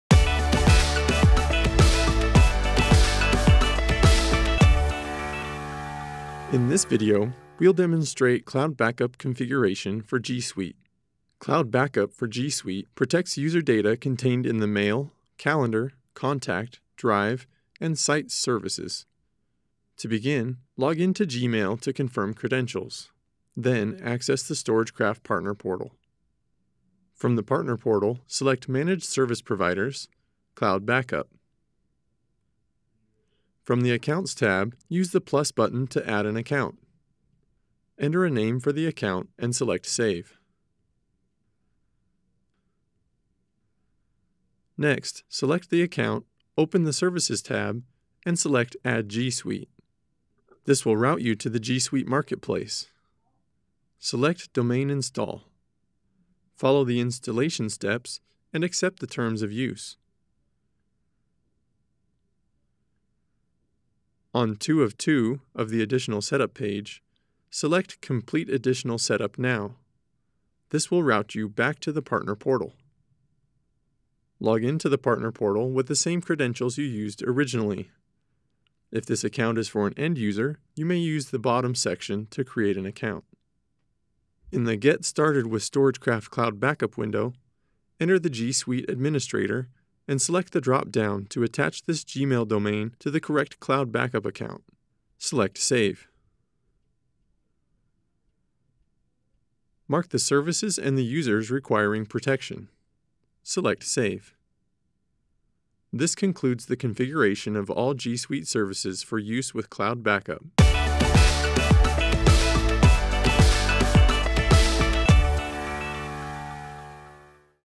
StorageCraft Cloud Backup Configuration for G Suite
In this video we’ll demonstrate how to configure G Suite protection using StorageCraft Cloud Backup.